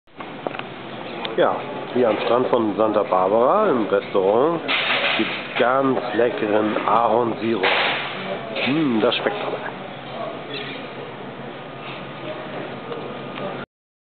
Delicious maple syrup in a restauant in Santa Barbara
Leckerer Ahornsirup in einem Restaurant in Santa Barbara.